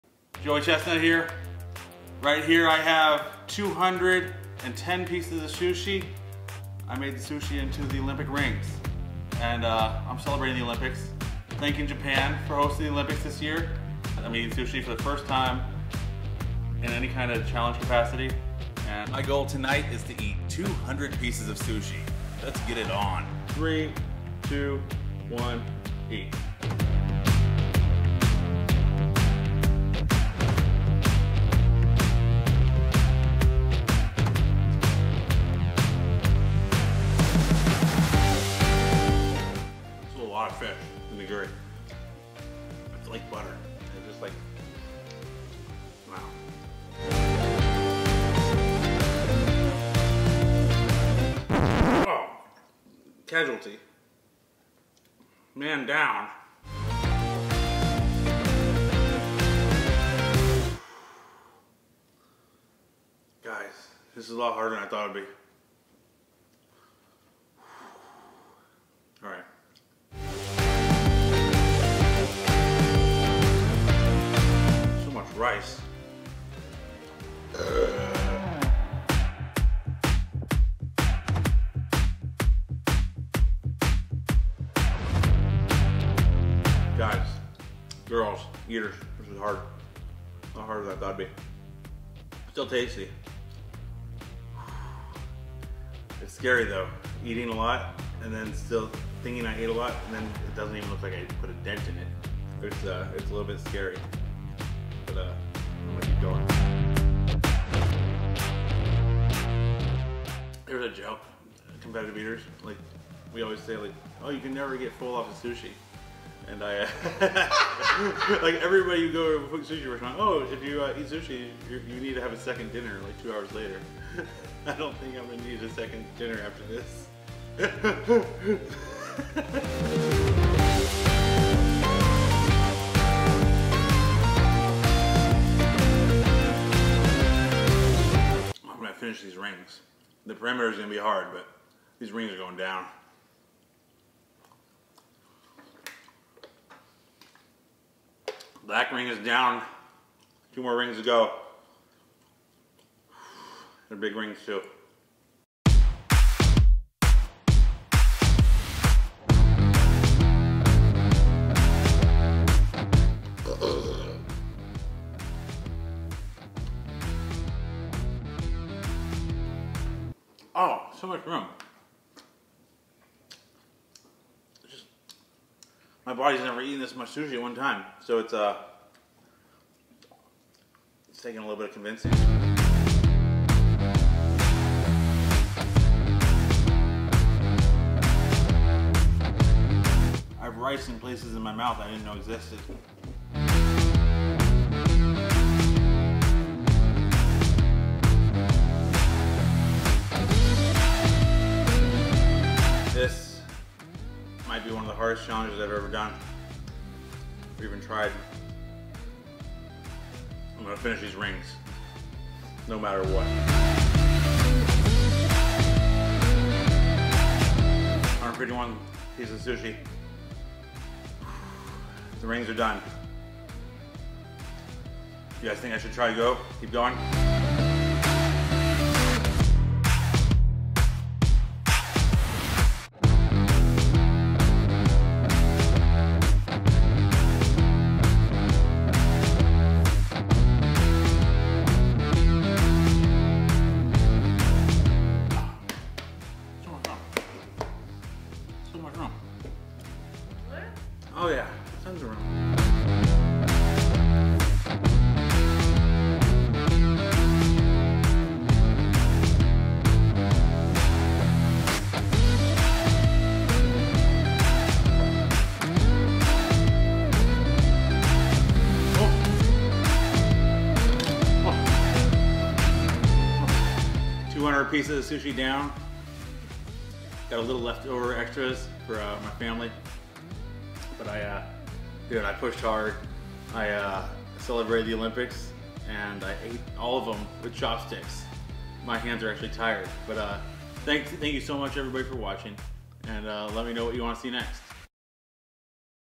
Celebrating The Olympics In Japan | Joey Chestnut Eats 200 Pieces Of Sushi
Hello big food lovers! I celebrated the end of the 2020 Olympics with an epic sushi platter in the shape of the Olympic rings.  This was fun to build and eat!!

Thank you Japan for hosting a beautiful Olympics!